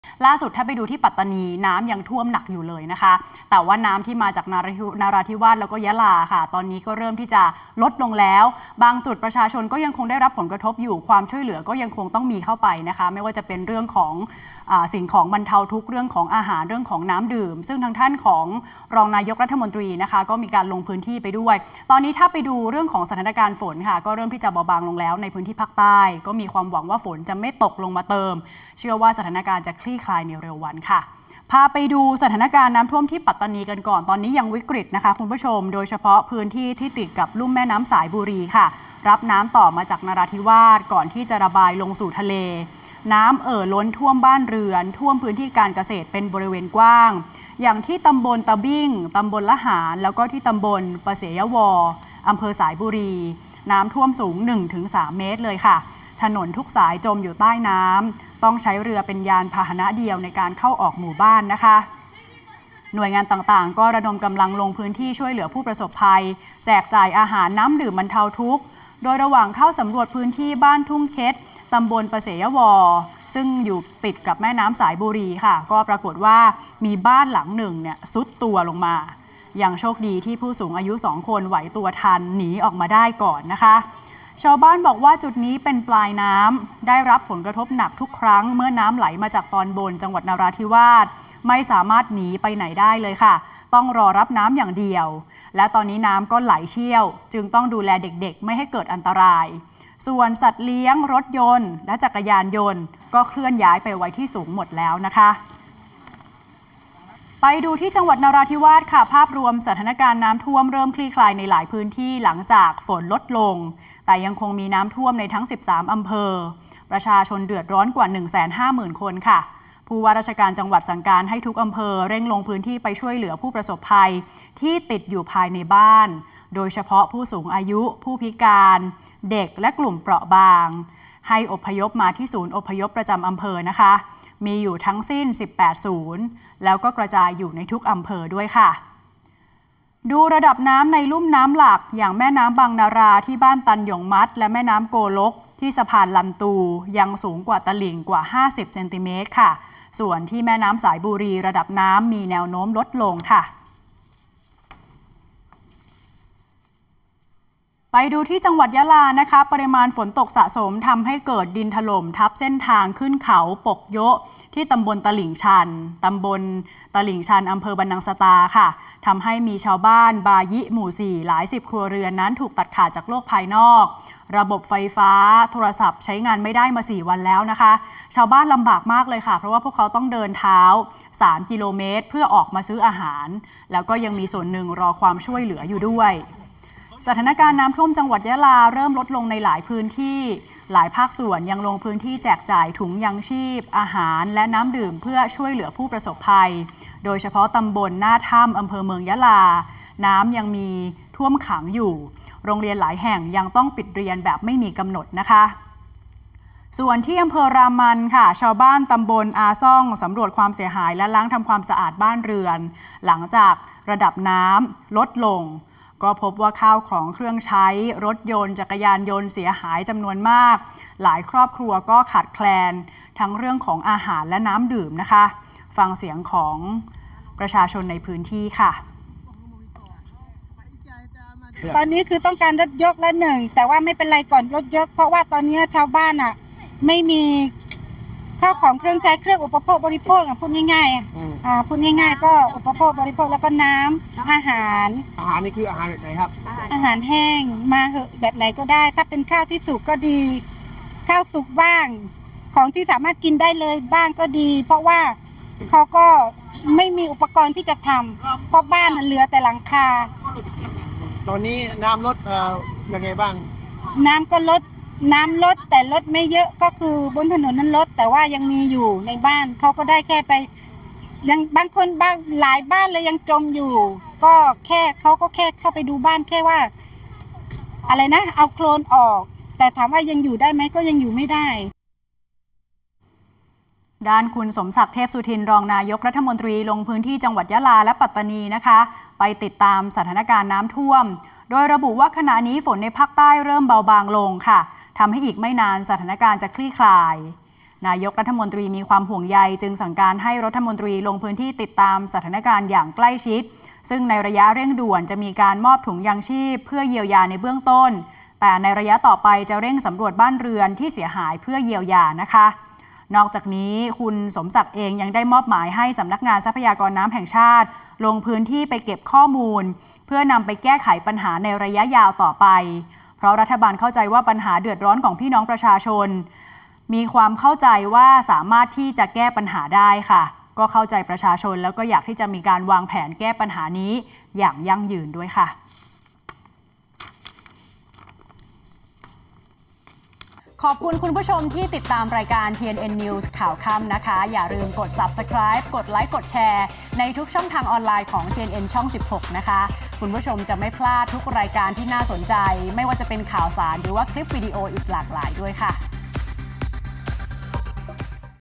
"สายบุรี" น้ำท่วมสูง 3 เมตร ยะลาดินถล่ม-นราฯยังจม 13 อำเภอ | TNN ข่าวค่ำ | 28 ธ.ค. 66
สถานการณ์น้ำท่วมที่ปัตตานี ริมแม่น้ำสายบุรีน้ำยังท่วมสูง 3 เมตร ชาวบ้านเดือดร้อนหนัก ถนนทุกสายจมอยู่ใต้น้ำ ต้องใช้เรือเป็นยานพหนะเดียวในการเข้า-ออกหมู่บ้าน  ส่วนที่จังหวัดยะลา เกิดดินถล่มทับเส้นทางขึ้นเขาปกโย๊ะ กระทบชาวบ้านที่ถูกตัดขาดโลภภายนอก 4  วันแล้ว  ด้าน จ.นราธิวาส ระดับน้ำเริ่มลดลงบ้างแล้วแต่ยังเดือดร้อนทั้ง 13 อำเภอ